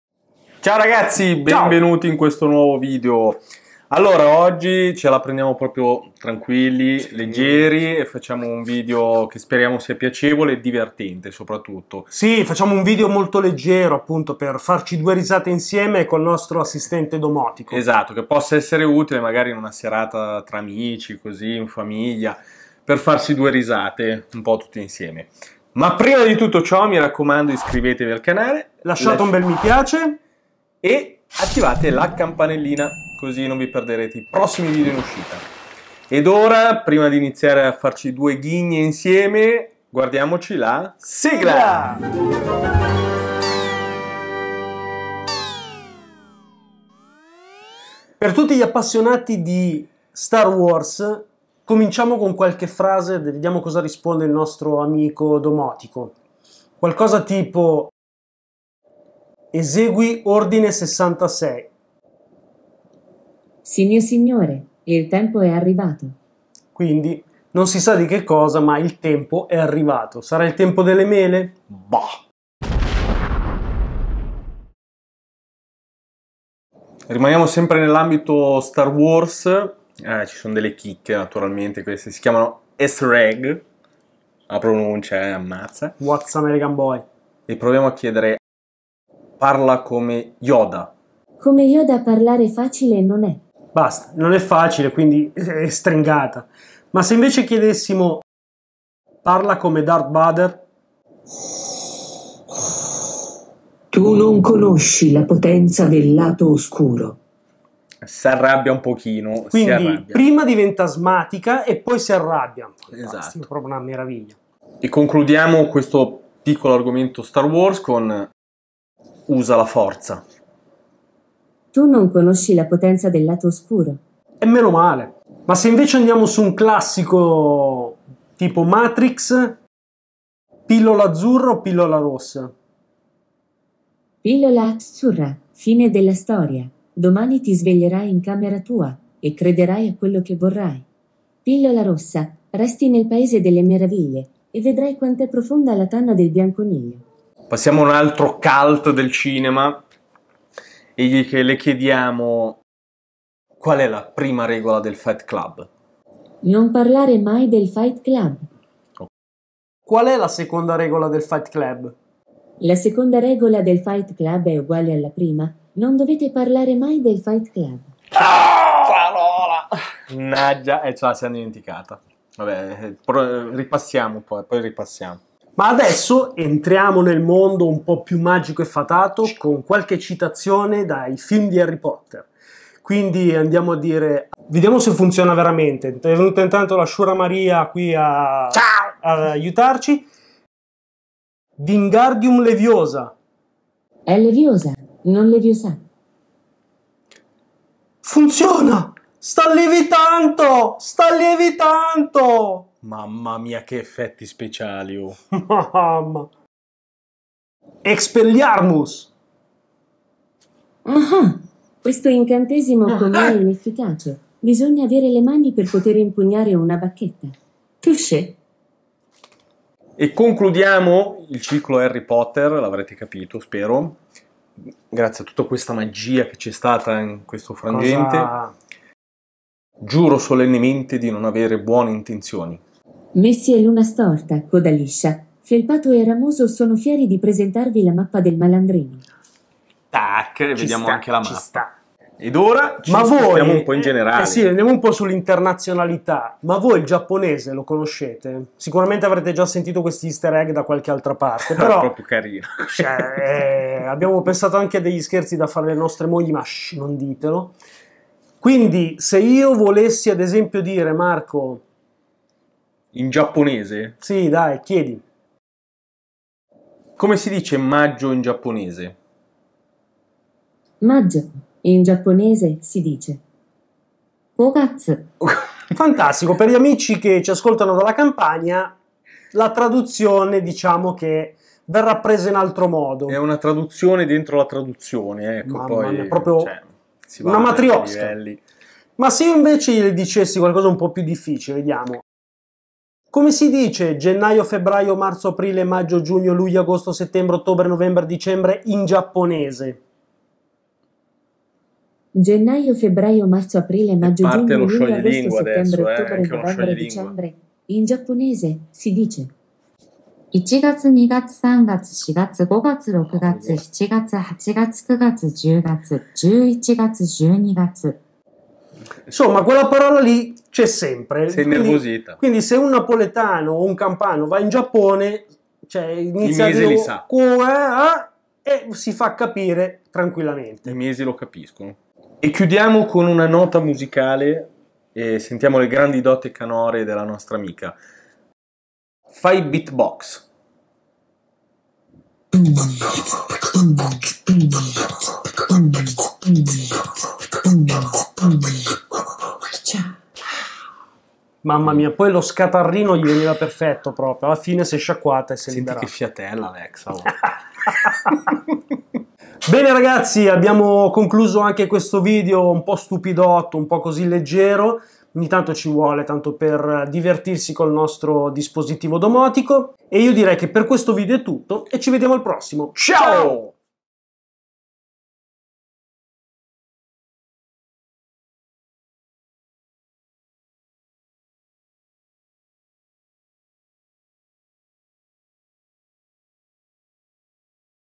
AMAZON ALEXA, FACCIAMOCI 2 RISATE CON LE SUE RISPOSTE ITA
Abbiamo trovato qualche domanda da fare ad Alexa, ma la parte divertente sono le risposte!